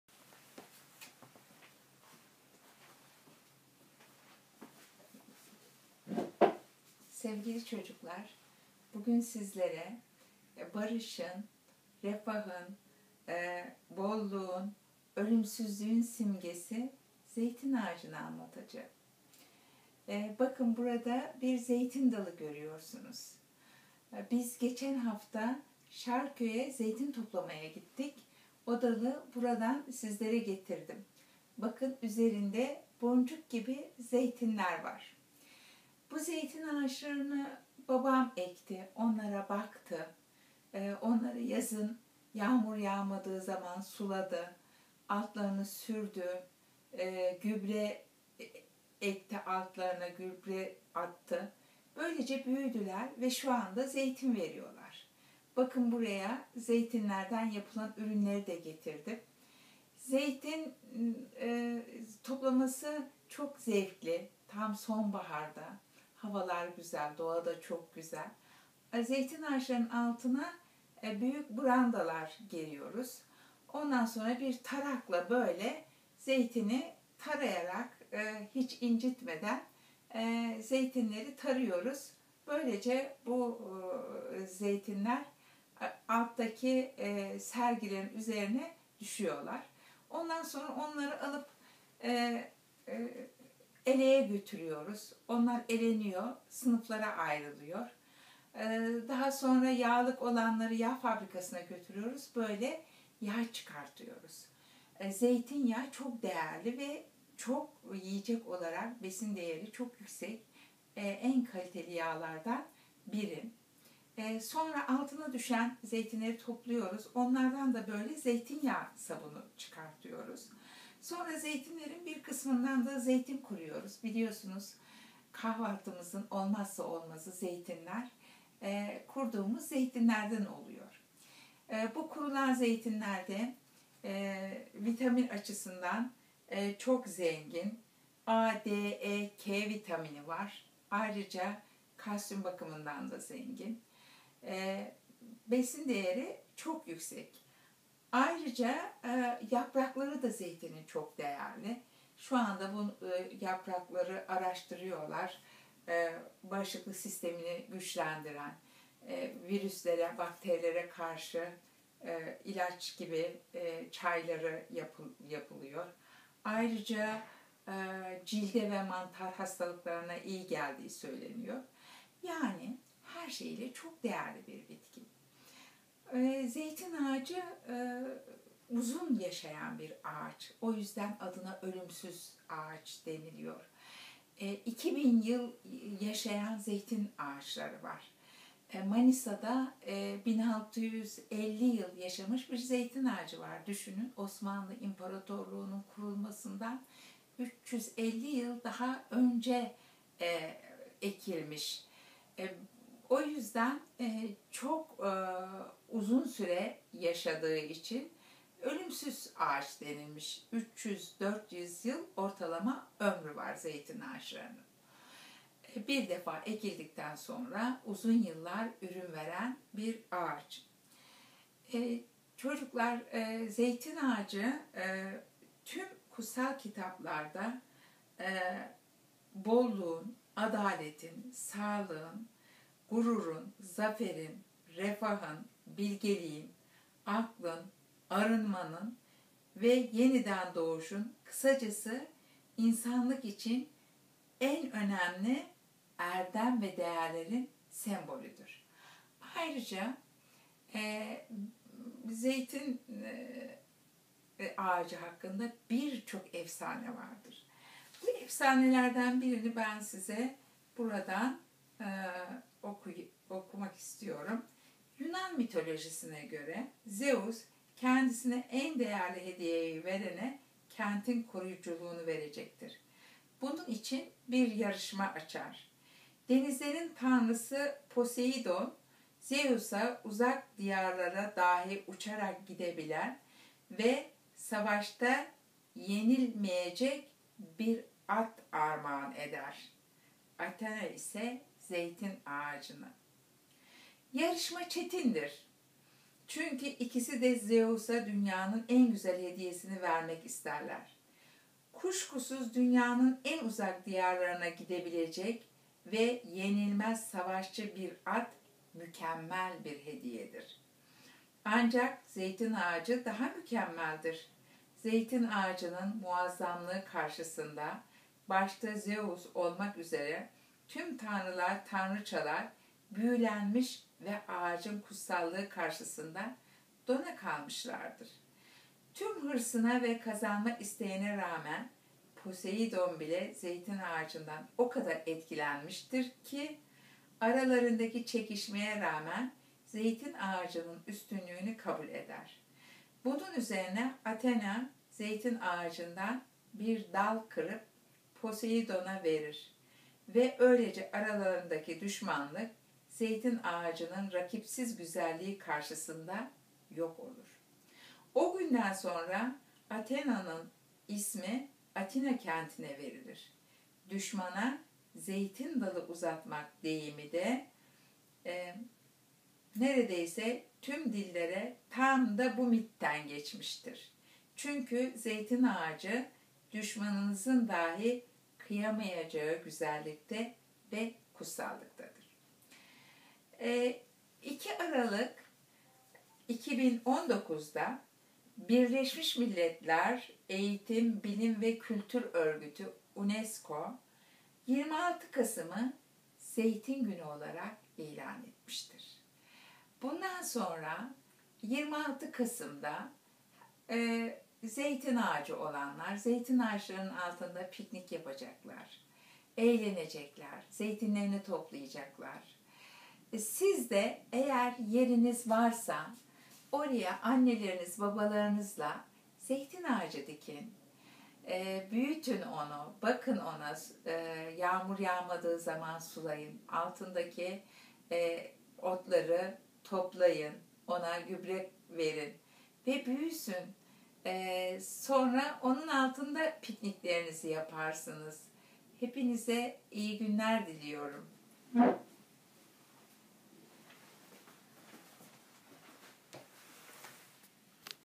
1. Sınıf ve tüm öğrencilere, zeytin ağacı v:7
Bu dersimizde öğrencilere, zeytin ağacını ve ürünlerini, doğa sevgisini anlatıyorum.